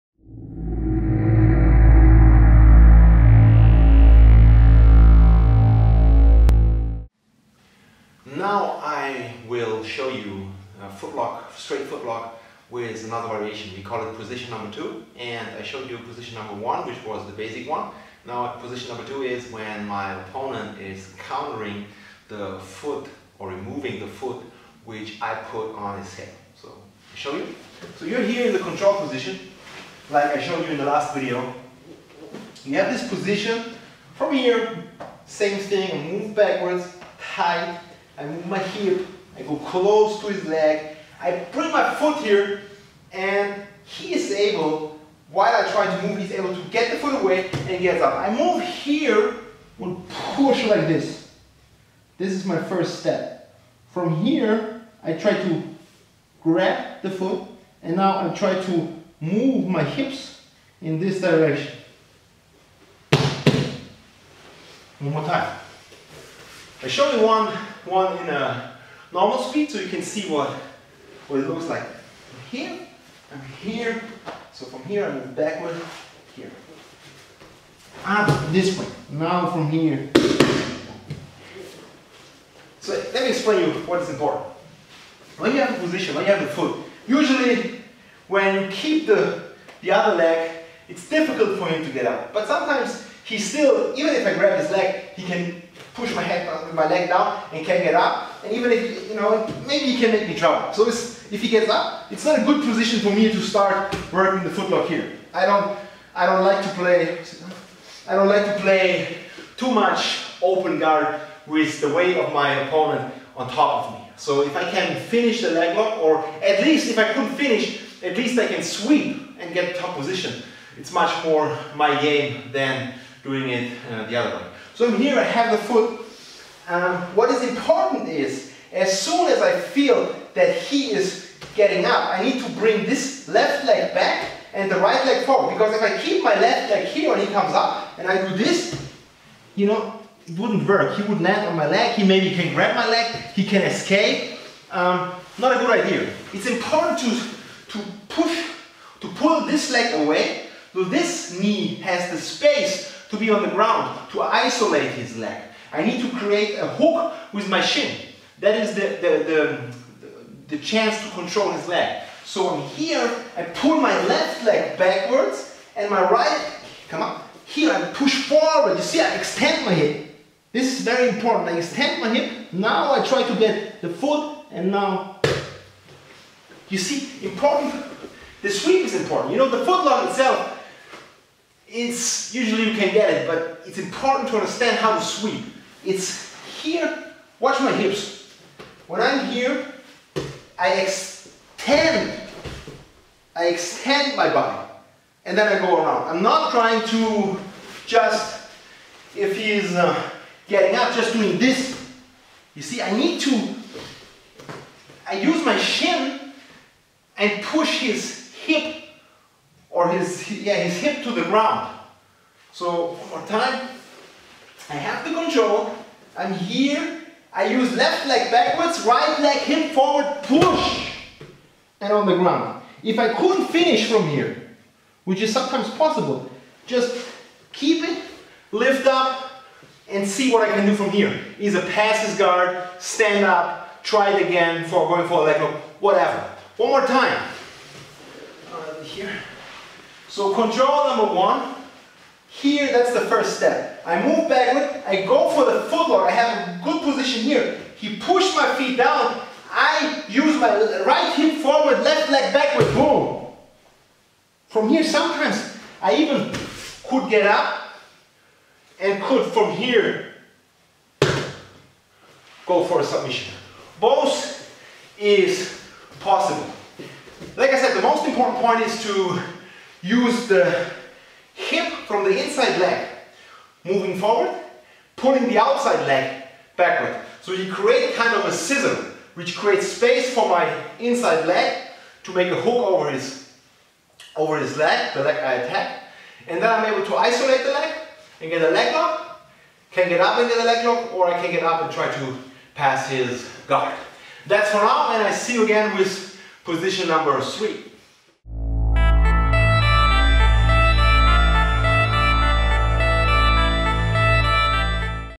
Mechanics of the Straight Foot Lock - Prevent the stand up
In this video BJJ Blackbelt Björn Friedrich shows how to finish the Straight Foot Lock when your opponent is trying to stand up.